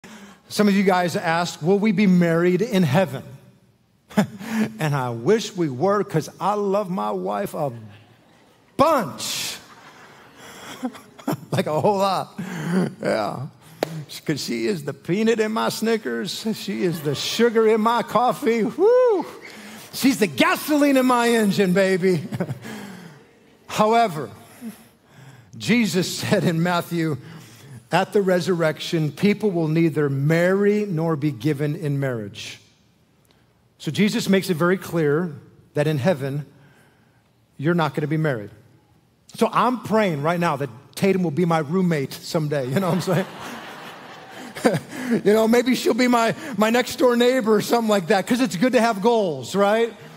You Ask. We Answer. | Will We Be Married In Heaven?
In this video, Pastor Chris answers the question: "Will We Be Married In Heaven?"

He sets up three ground rules:

1. We do not waver on what the Bible clearly states.
2. If the Bible doesn't answer a question, we apply biblical principles.
3. If we can't find biblical principles, we give our opinion.